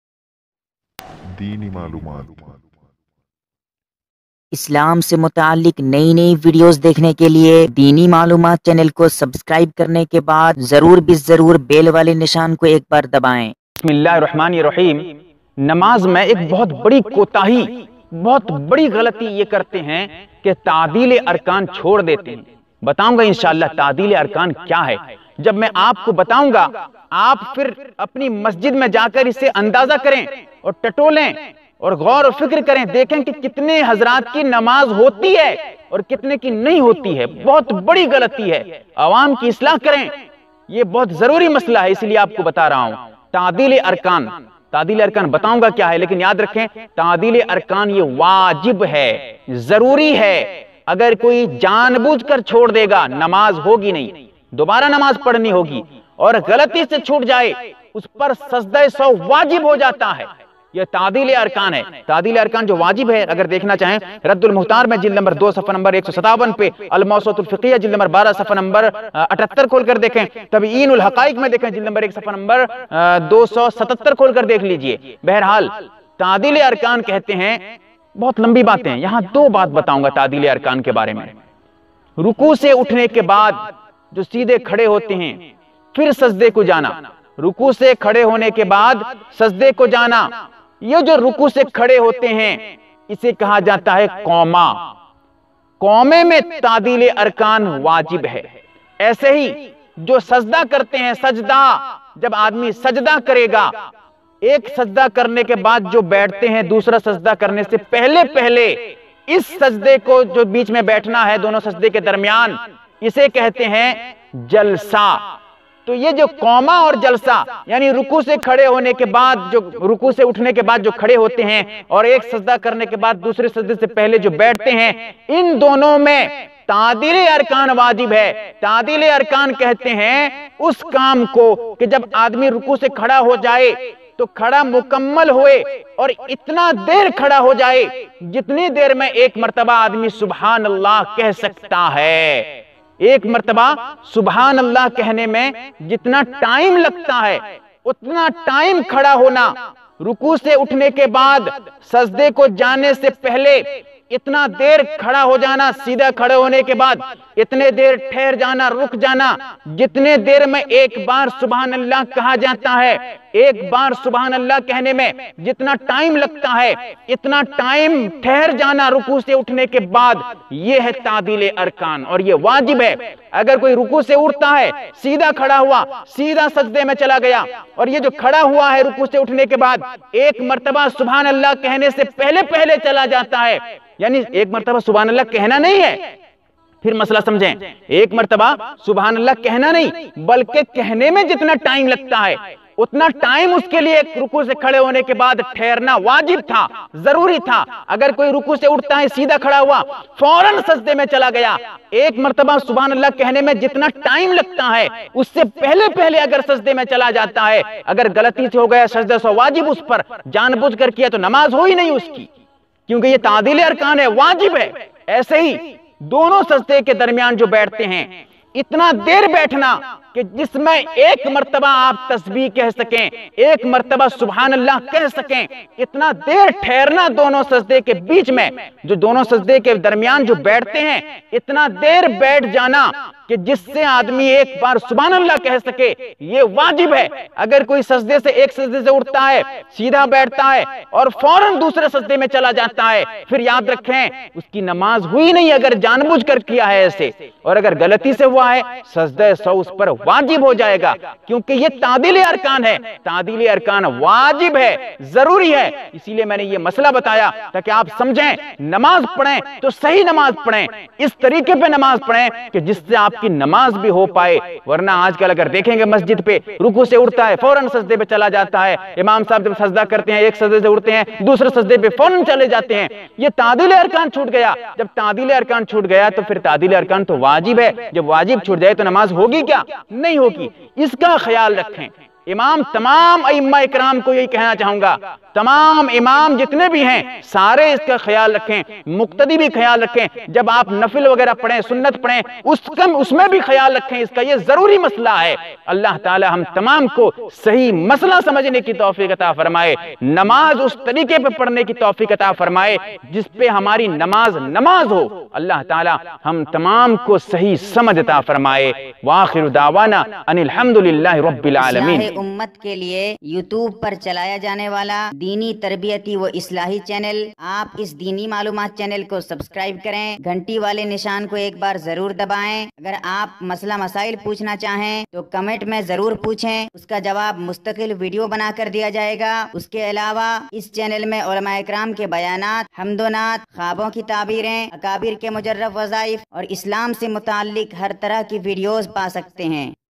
Namaz mein Tadeel e Arkaan | Namaz ka tarika | Namaz ka sahi tarika | تعدیل ارکان | نماز
Isme namaz ke mutaliq ik bahut badi galati ki islaah ki gai hai or namaz ka sahi tariqah bataya gaya hai aaj kal namaaz me ik badi galati ye hoti hai ke tadeel e Arkaan ada nahi karte hain isme ye bataya gaya hai ke tadeel e arkaan h kya uska kya hukum hai tafseeli baat pesh ki gai hai,

Channel by

نمبر :- ٠٨٩٠٨٩٣٩٣٤٣

          دیوبند دیوبند